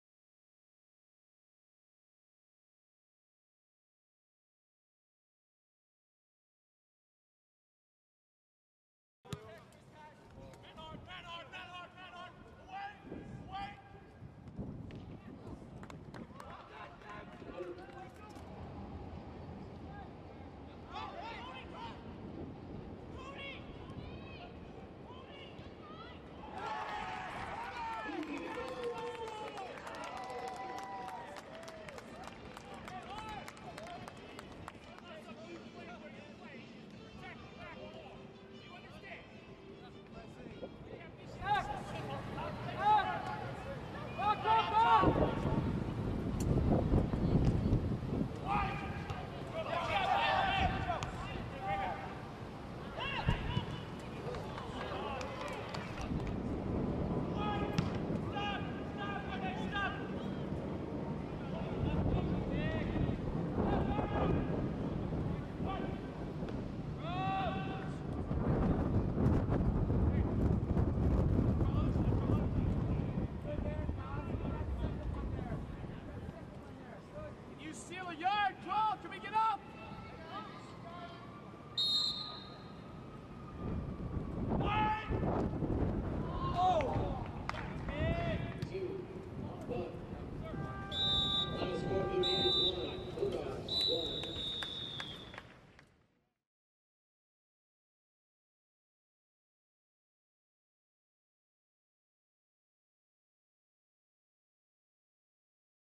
DePaul Men's Soccer vs. Drake
Highlights from DePaul's 1-1 draw with Drake